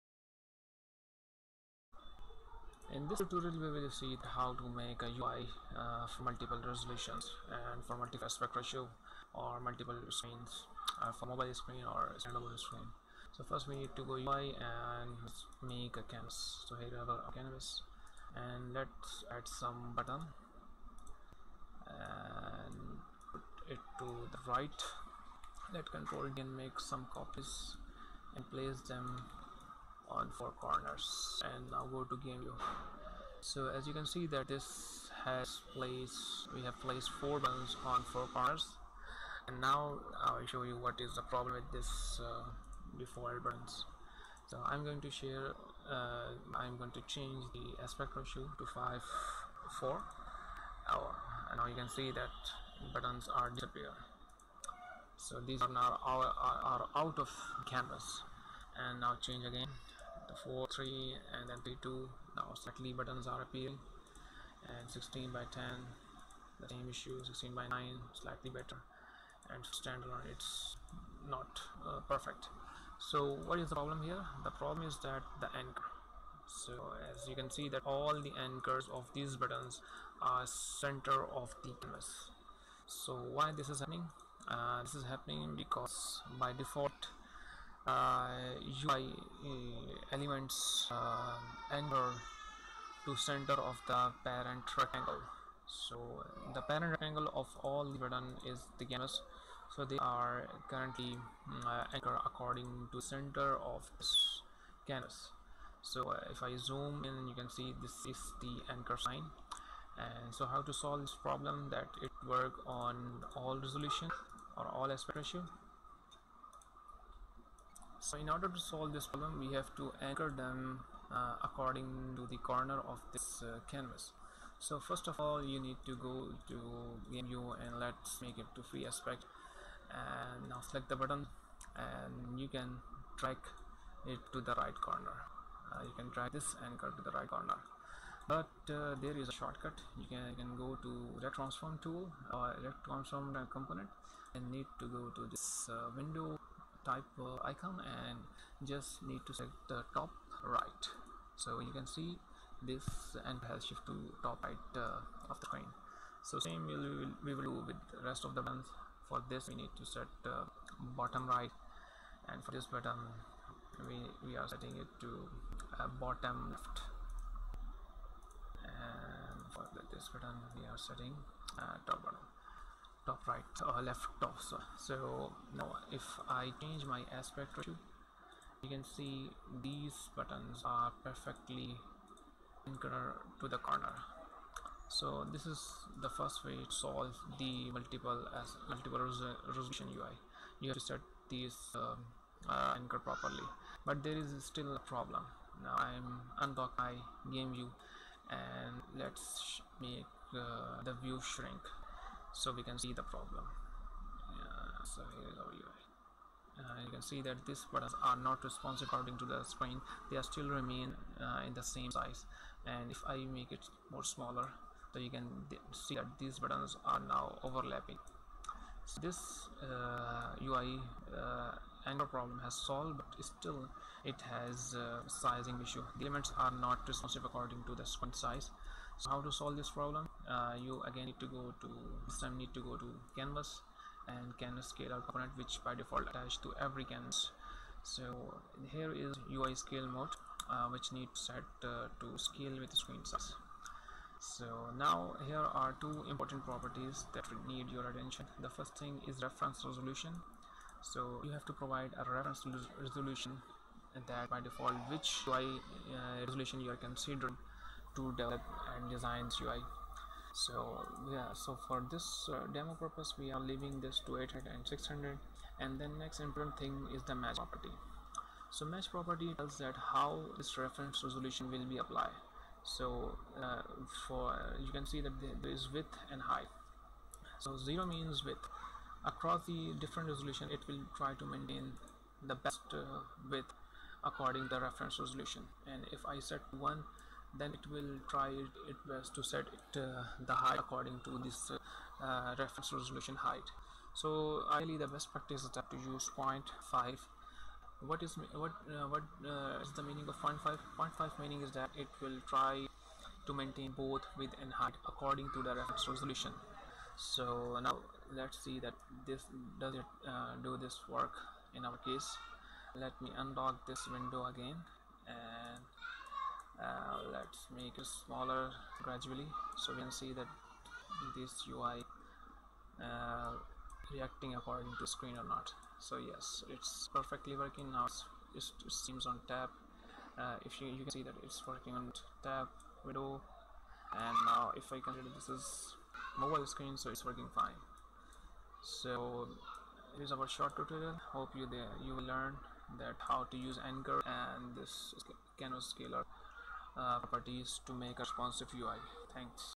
Designing UI for Multiple Resolution and Aspect ratios - How to make responsive UI in Unity3d
In this beginners unity tutorial, you learn How to make a responsive UI in unity3d.  Learn how to design multiple resolutions or multiple aspect ratio User interface (UI) in Unity Game-engine using Unity UI Canvas.